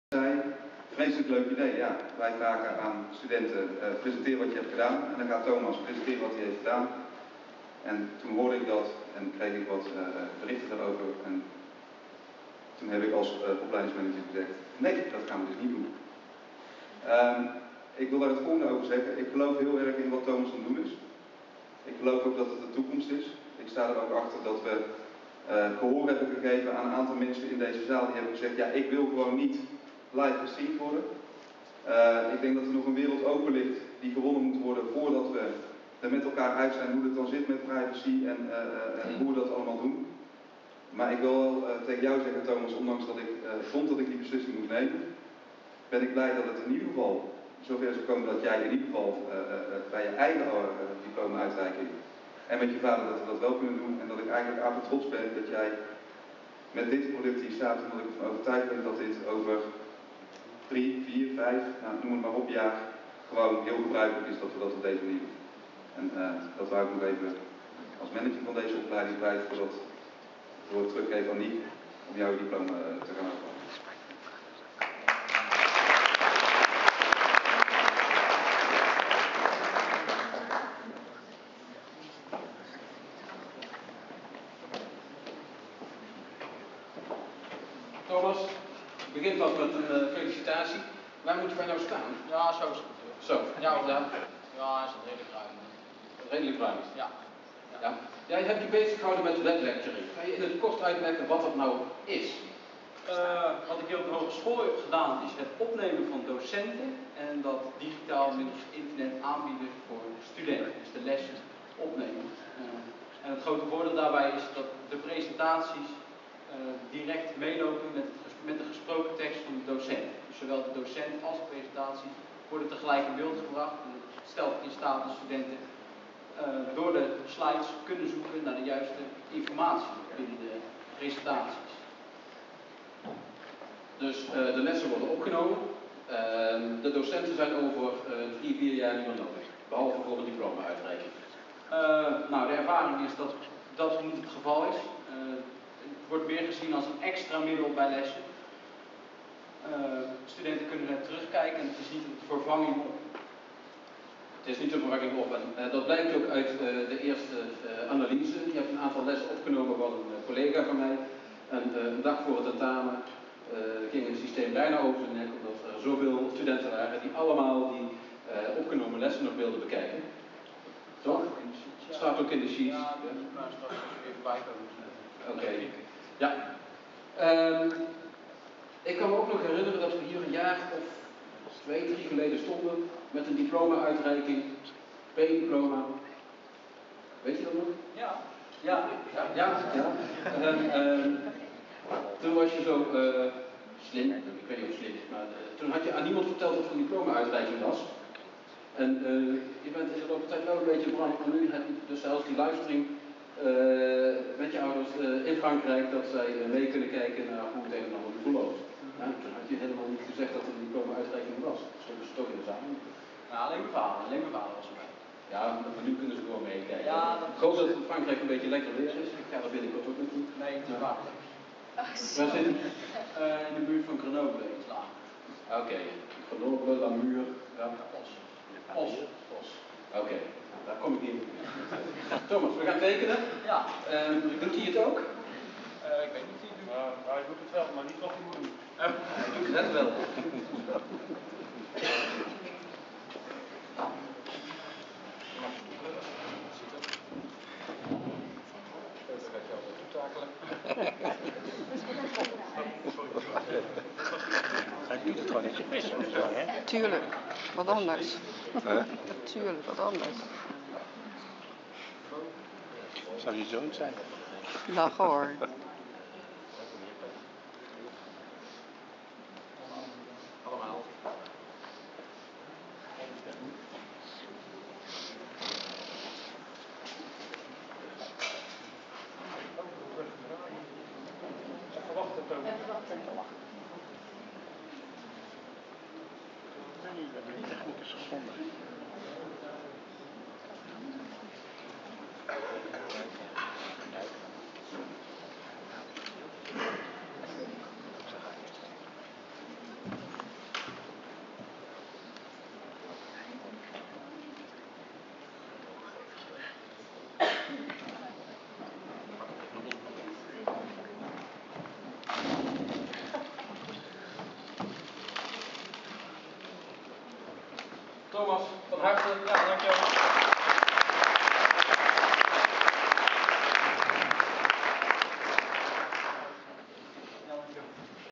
BSc diploma Informatica voor Thomas Uittenbogaard
Op vrijdag 7 juli 2012 kreeg Thomas Uittenbogaard zijn BSc diploma Informatica op de Hogeschool Leiden.
Thomas is afgestudeerd op Videolectures, waarmee hij diverse colleges op de Hogeschool Leiden heeft opgenomen.
Als bonus heeft hij zijn eigen diploma-uitreiking opgenomen. Deze opname was als live-stream te volgen voor belangstellenden, maar vooral voor zijn vader Henk, die in Frankrijk in een soort Tour de France meereed, en daardoor niet aanwezig kon zijn.
De opname begint met een toelichting van de opleidingsdirecteur Patrick over het opnemen van colleges en van deze diploma-uitreiking.